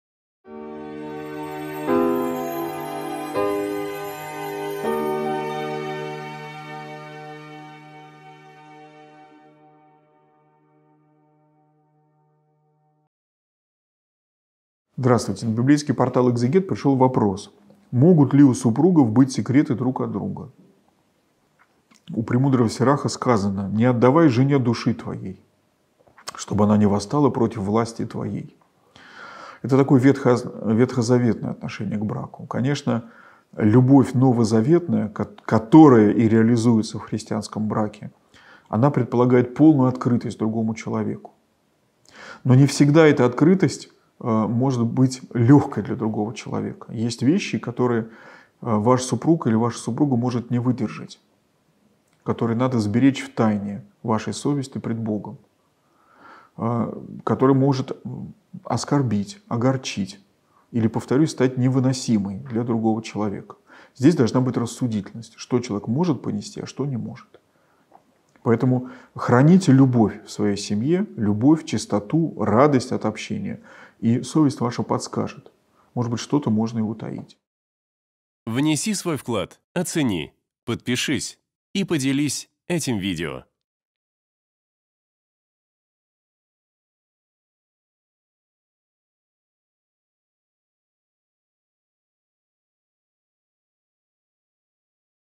Могут ли у супругов быть секреты друг от друга? Протоиерей Феодор Бородин
В сегодняшнем видео отец Феодор Бородин отвечает на вопрос: "Могут ли у супругов быть секреты друг от друга?".

🔸 Доверие.
🔸 Новозаветная любовь.
🔸 Совесть пред Богом.
🔸 Рассудительность в семье.

📜 Читать книга Премудрости Иисуса, сына Сирахова глава 9 стих 2 (с толкованиями)